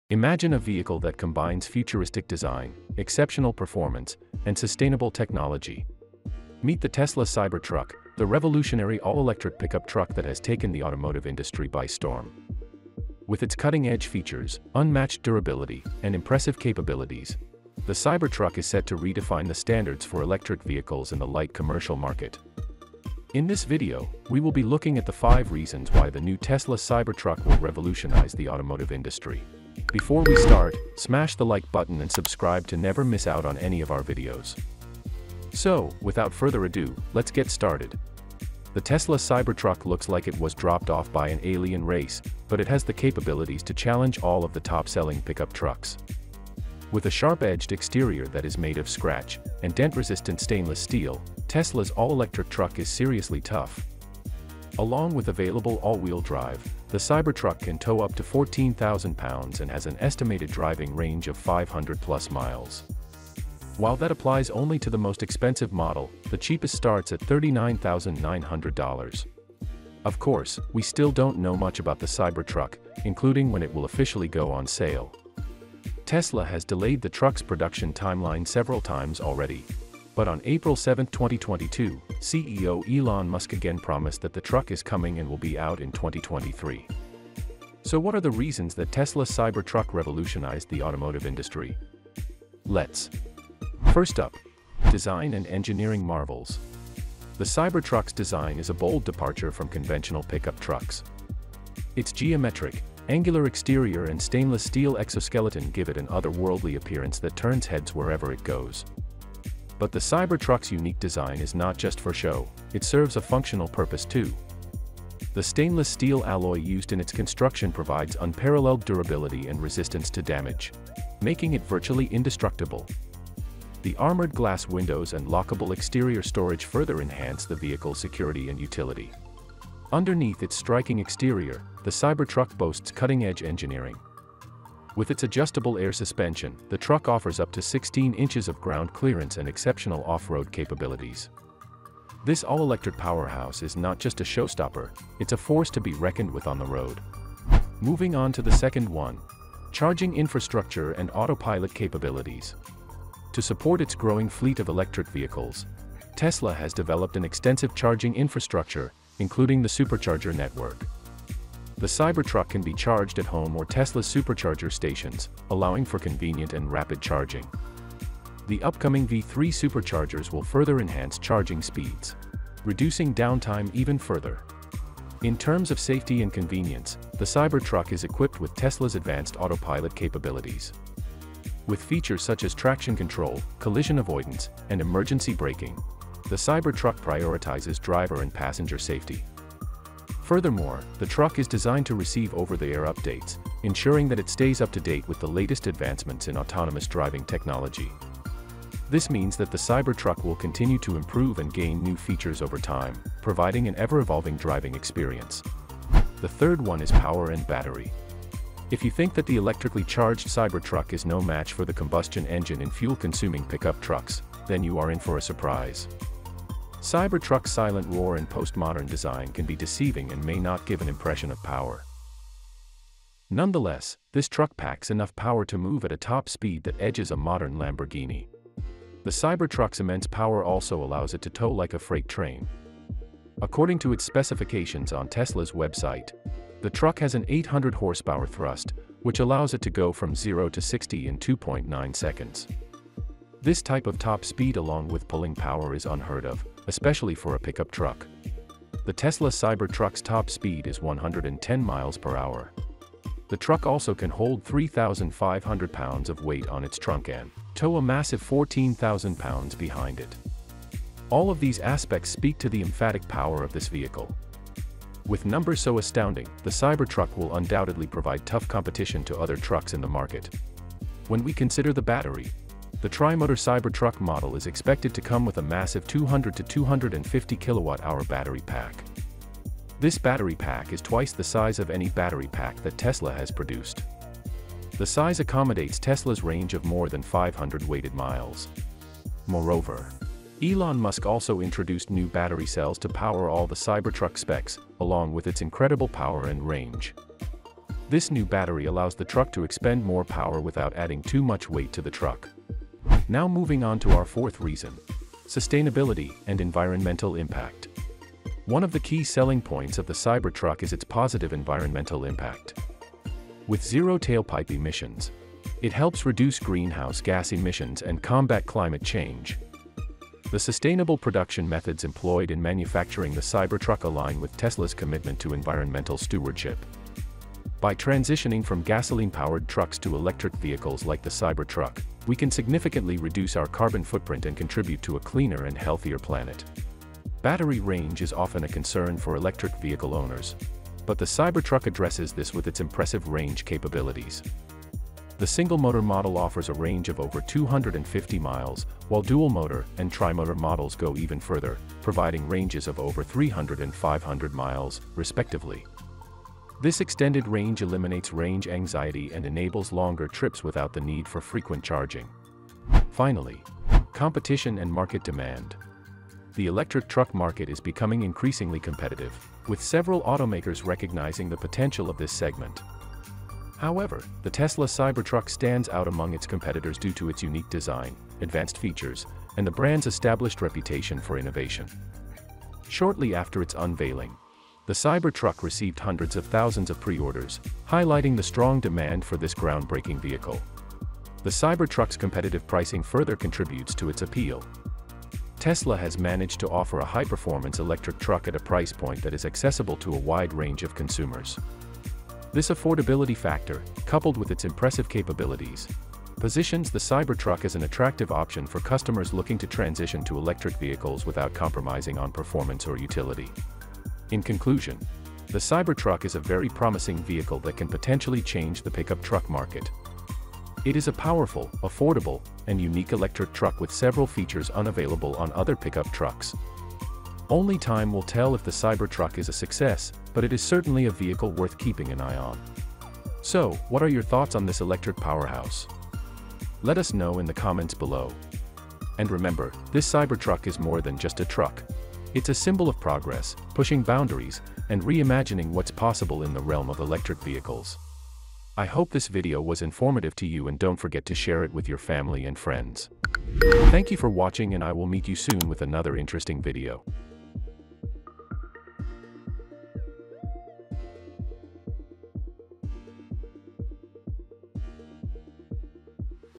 5 Reasons Why the New Tesla Cybertruck Will Revolutionize the Automotive Industry
Are you curious about the new Tesla Cybertruck? If so, check out this video to find out 5 reasons why the new Tesla Cybertruck will revolutionize the automotive industry.

From Tesla's advanced safety features to their cutting-edge design, this truck is sure to change the way we think about automotive transportation. Get a sneak peak at this impressive truck and find out why it's sure to revolutionize the automotive industry!
Video Titel: 5 Reasons Why the New Tesla Cybertruck Will Revolutionize the Automotive Industry